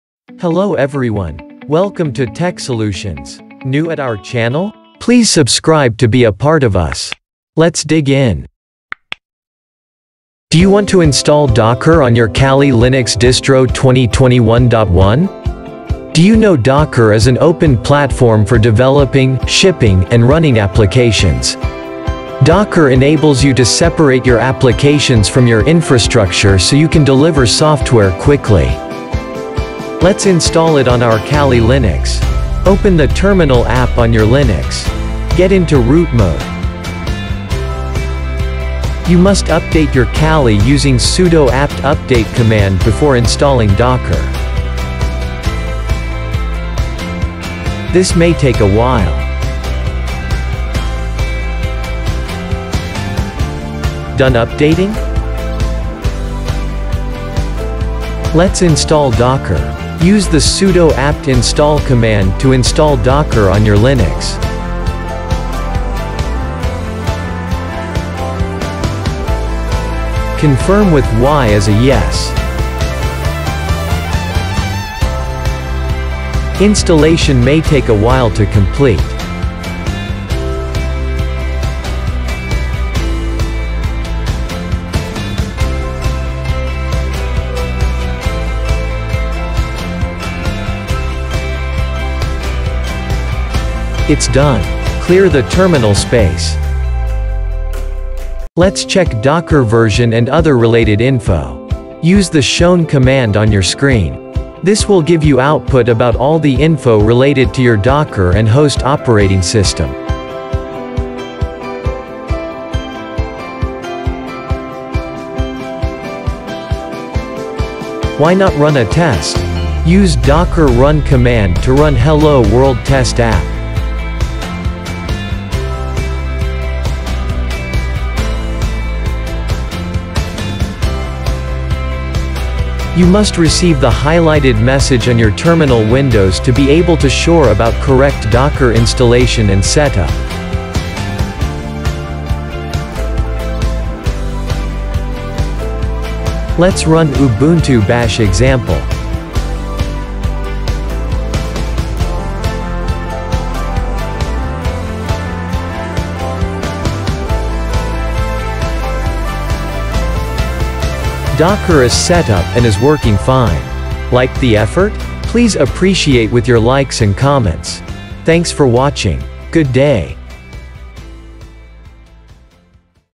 How to Install Docker on Kali Linux 2021.1 | Install Docker Engine on Linux | Docker.io Container
Docker is an open platform for developing, shipping, and running applications. Docker enables you to separate your applications from your infrastructure so you can deliver software quickly. With Docker, you can manage your infrastructure in the same ways you manage your applications. By taking advantage of Docker’s methodologies for shipping, testing, and deploying code quickly, you can significantly reduce the delay between writing code and running it in production. You can download and install Docker on multiple platforms.